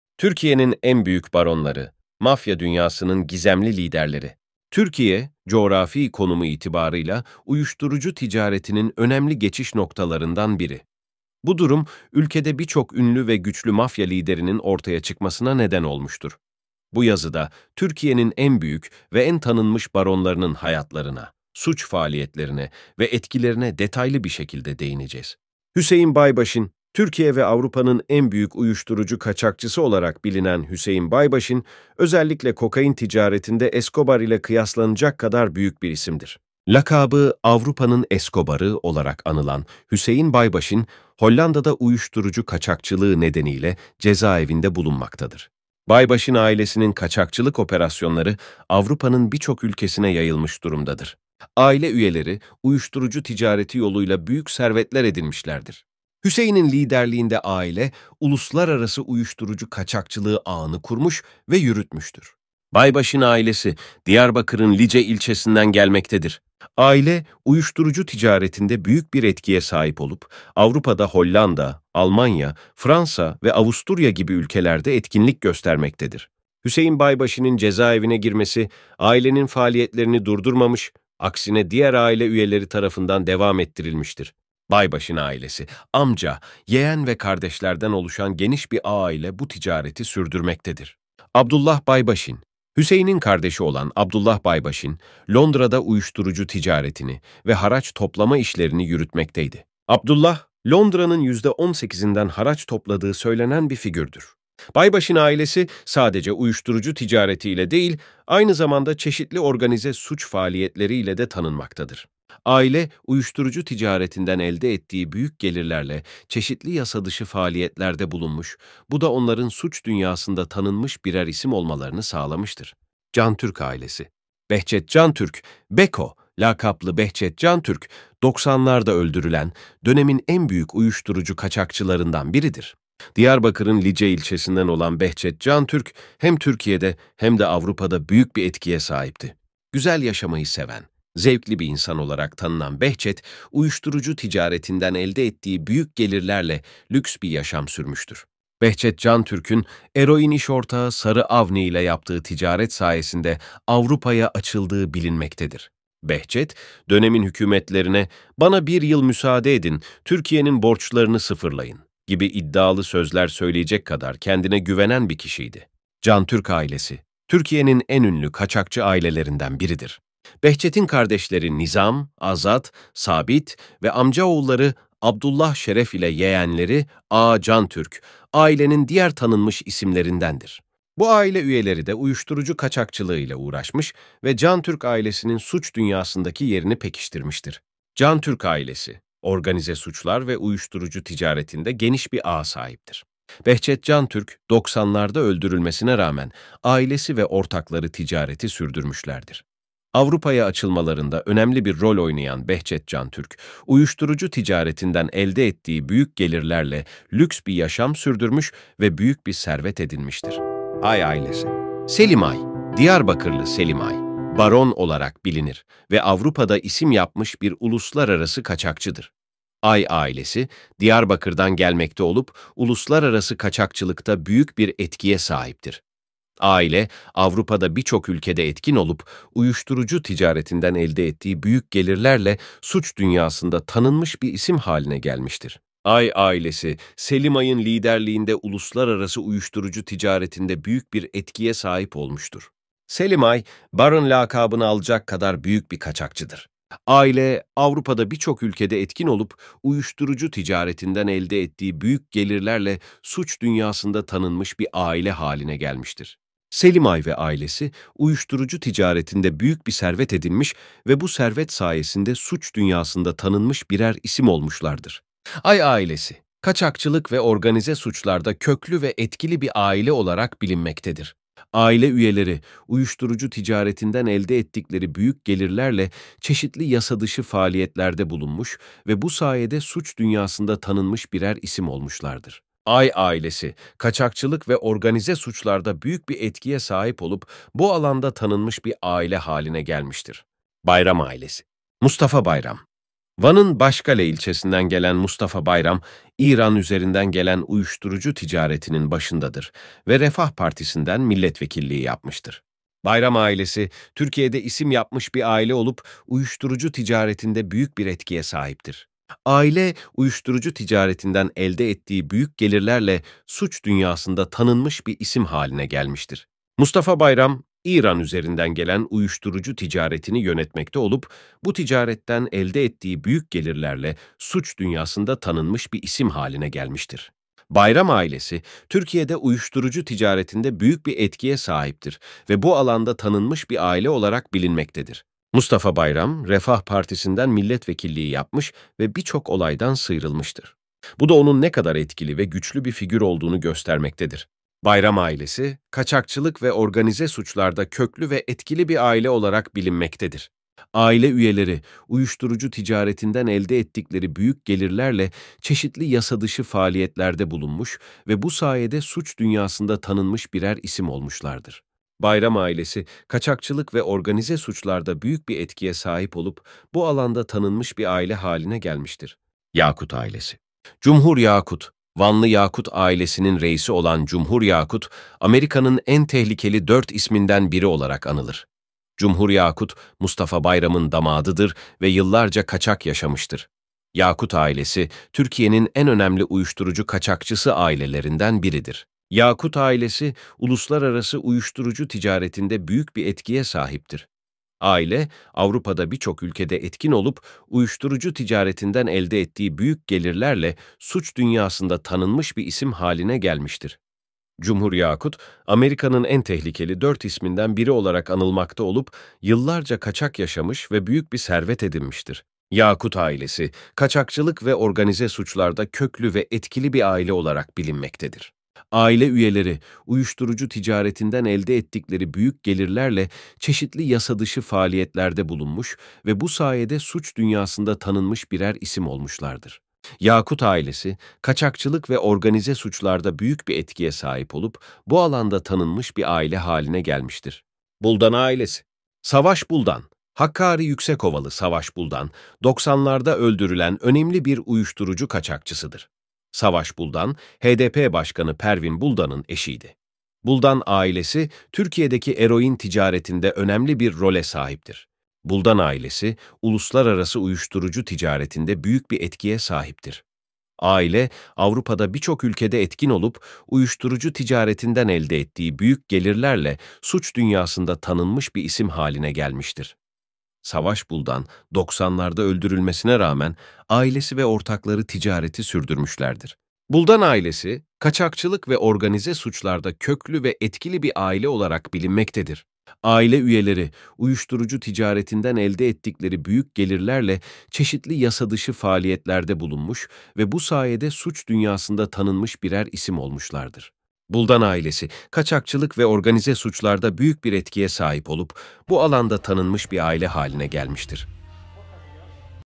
Türkiye'nin En Büyük Baronları : Baronların Gerçek Hikayeleri
Türkiye'nin En Büyük Baronları :  Baronların Gerçek Hikayeleri     (Part 1 )

Türkiye'nin  Kaçakçılığı ve Ünlü Aileler: Türkiye, doğudan gelen uyuşturucunun geçiş güzergahında yer alır ve  kaçakçılığı, büyük aileler ve aşiretler üzerinden gerçekleşir. Ünlü baron aileleri arasında Baybaşin, Cantürk, Soytaş, Ay, Bayram, Yakut, Buldan, Işık, Daş, Stoier, Güven, Çetinkaya, Yavaş, Han, Hacı Süleymanoğlu (Oflu İsmail), Halil Havar, Abuzer Uğurlu ve Arnavut Samir bulunur.